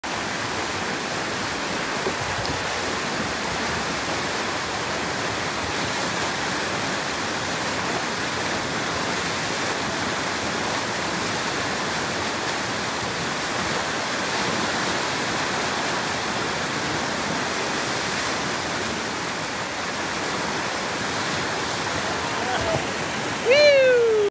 Tandem Side Surf
Tandem side-surf action on one of our '4 Rivers in 4 Days' whitewater canoe trips.